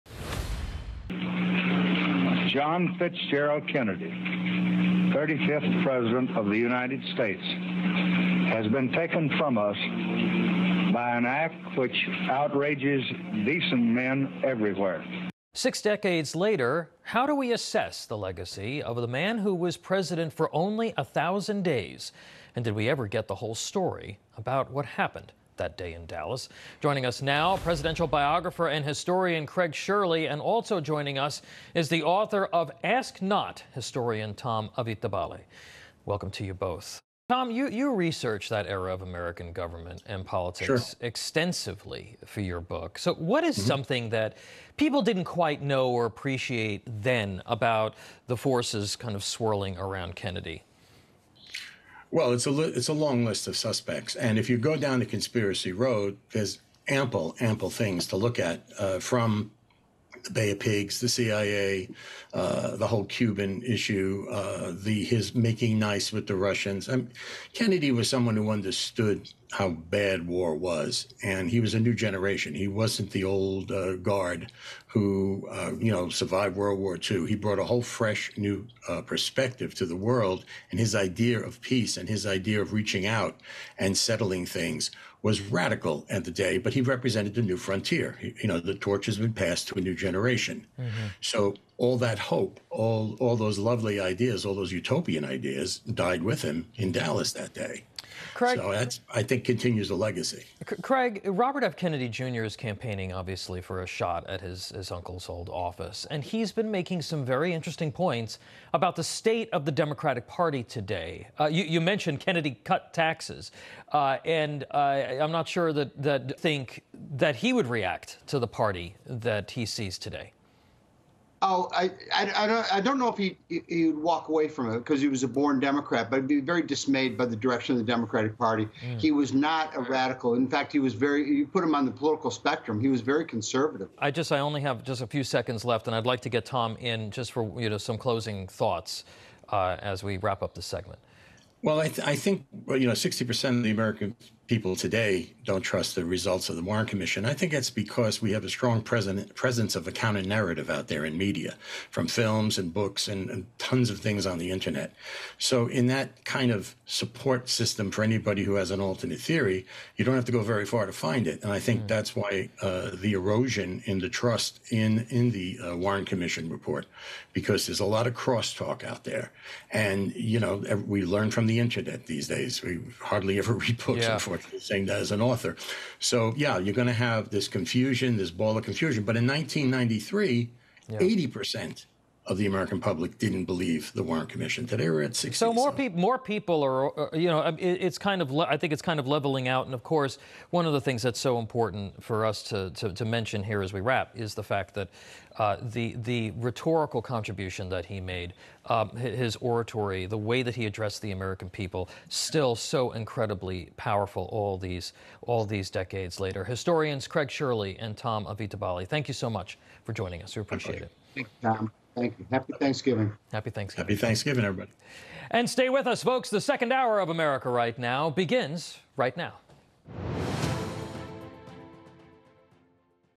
TOM IN THE NEWS 11 25 23
A panel discussion on national news about JFK's Legacy and public perceptions on the 60th Anniversary of his assassination.